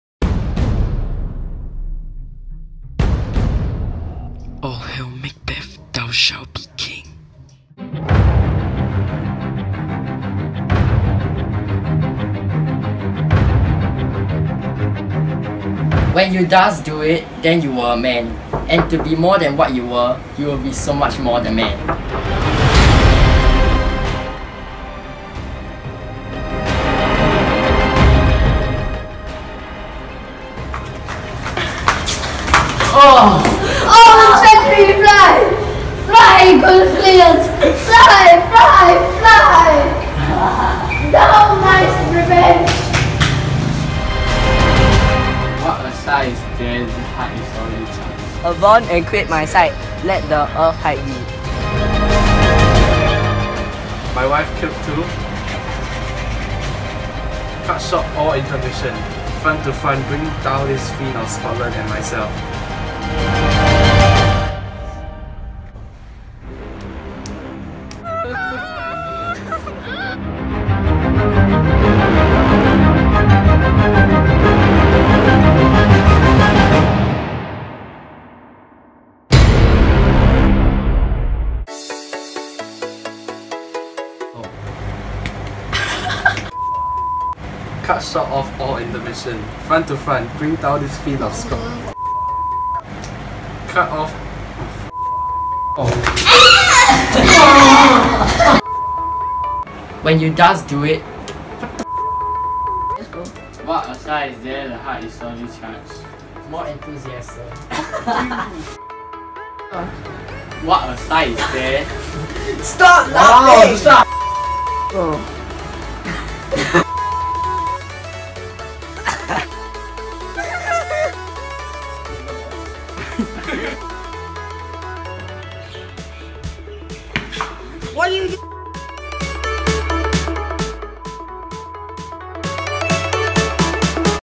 Macbeth (Parody Trailer)
SHSS Lit Project 2016
Members: Jarren, Joel, Joshua, Thaddaeus, Xian Xin

Joshua as Macbeth, the Gentlewoman & a murderer
Jarren as Lady Macbeth, Fleance & the witch narration
Xian Xin as Duncan, the doctor & a murderer
Thaddaeus as Banquo and Malcolm
Joel as Macduff and a murderer

Script Writing Done by Jarren
Editing done by Jarren